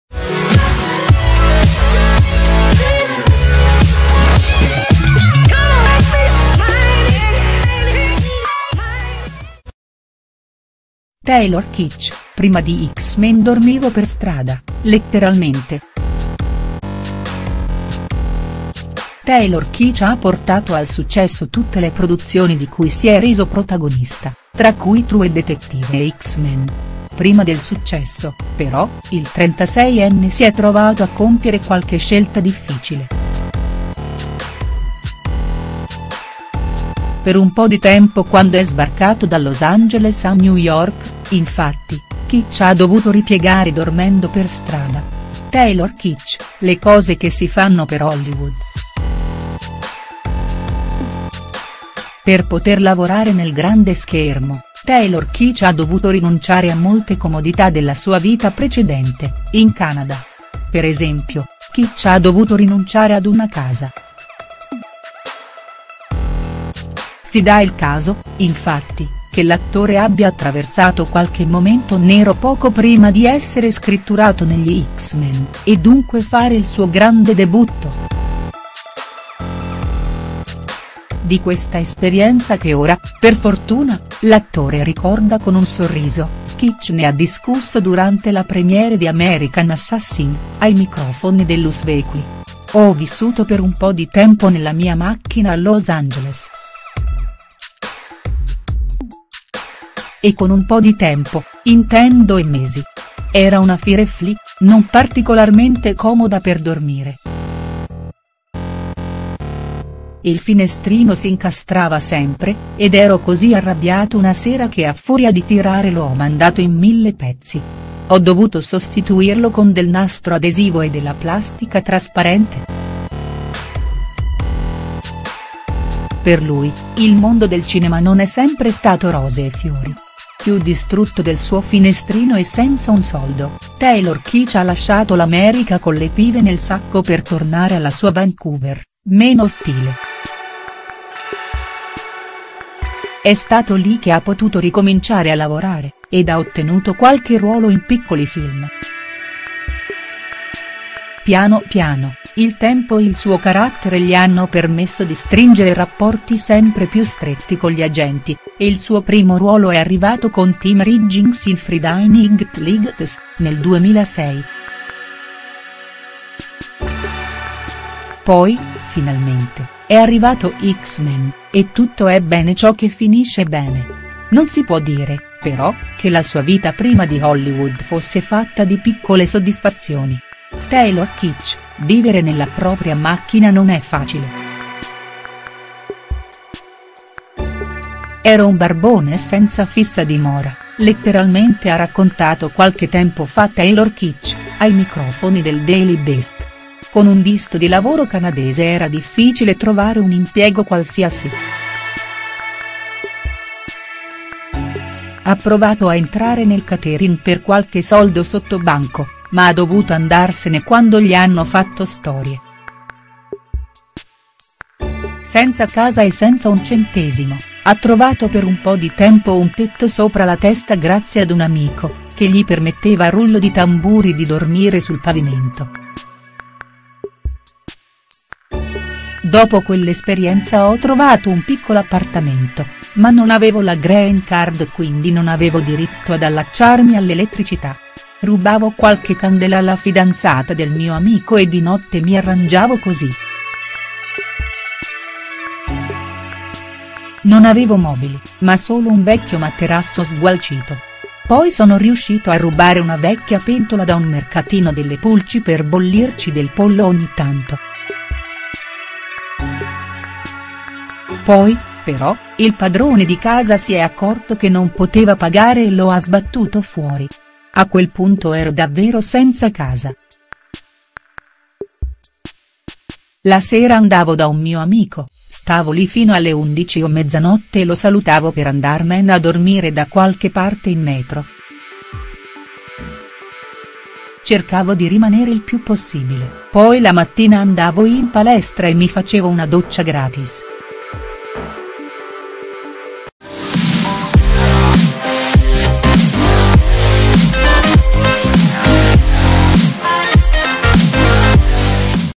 Taylor Kitsch: "Prima di X-Men dormivo per strada (letteralmente)".HD
Al momento, Taylor Kitsch si gode una splendida casa e una carriera d’attore di successo, pronto a imbarcarsi in nuovi progetti ogni giorno. Oggi, Taylor Kitsch può dire di no ad un copione che non è di suo gradimento. Per lui, però, la vita non è sempre stata così favorevole.